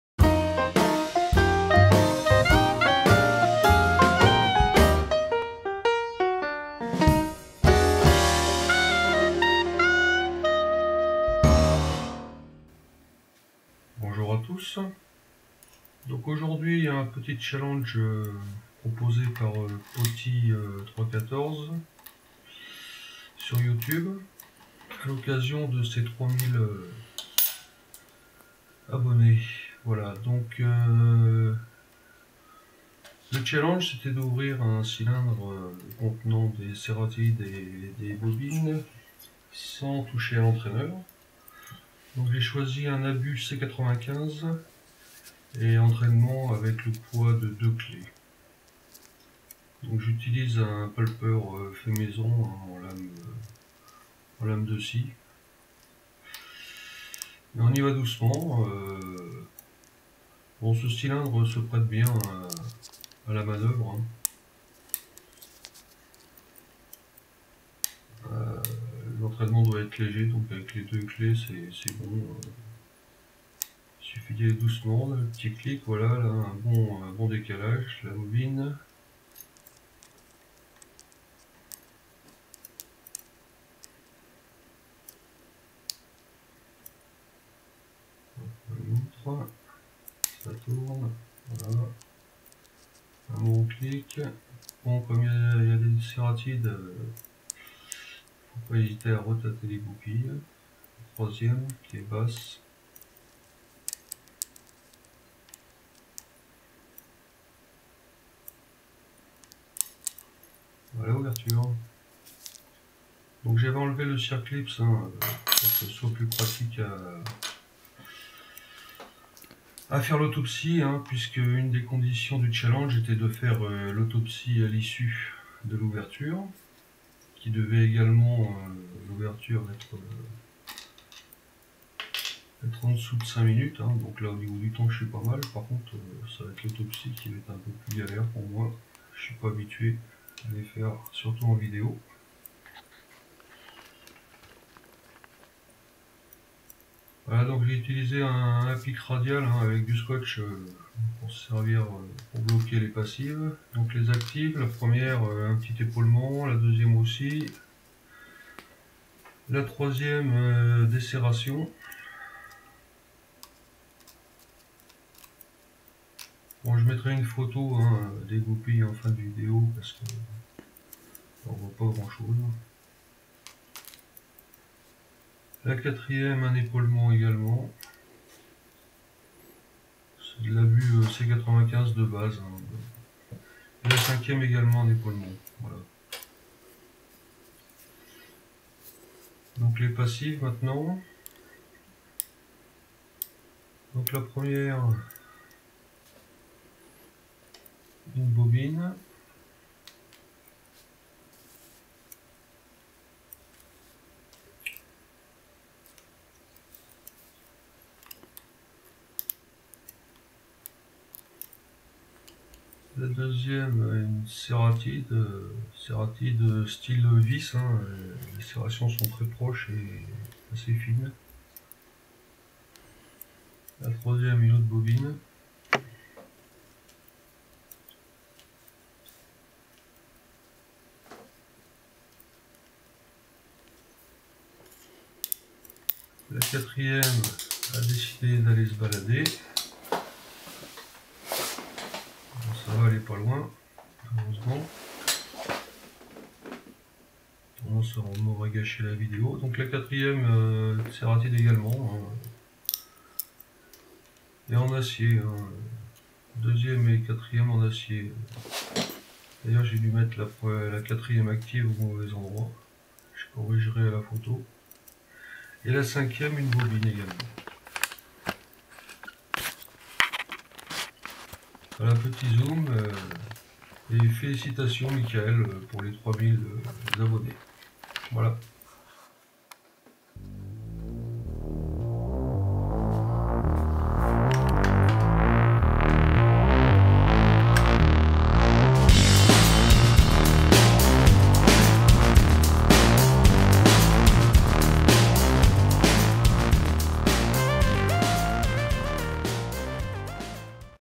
Challenge Potti314: Abus C95 à une main
A l'occasion des 3000 abonnés à sa chaine Youtube, Potti314 a lancé un petit challenge:
Ouvrir en moins de 5 min un cylindre comportant au moins une serrated et une bobine, sans toucher l'entraineur et faire l'autopsie dans la foulée: voici ma contribution.